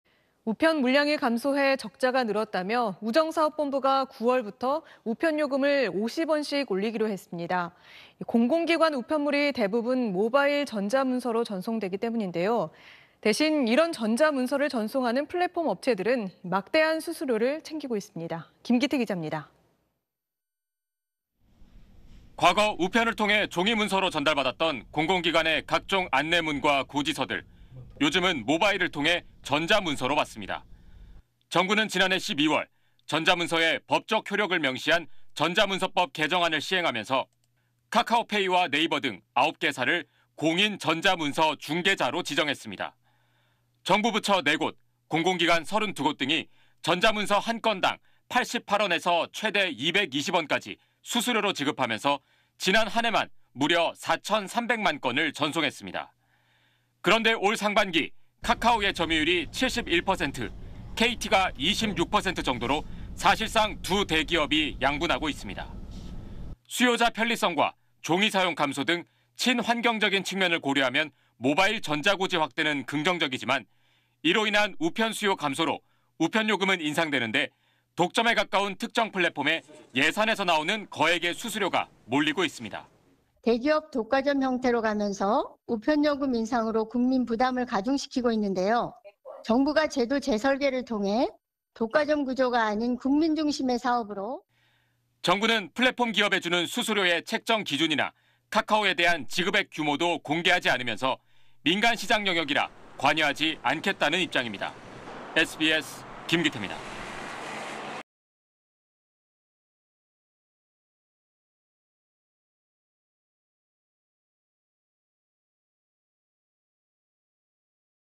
우편 요금 '50원 인상'…뒤에서 웃는 업체들이 있다? / SBS
우편 물량이 감소해 적자가 늘었다며 우정사업본부가 9월부터 우편 요금을 50원씩 올리기로 했습니다. 공공기관 우편물이 대부분 모바일 전자문서로 전송되기 때문인데요, 대신 이런 전자문서를 전송하는 플랫폼업체들은 막대한 수수료를 챙기고 있습니다.
김기태 기자입니다.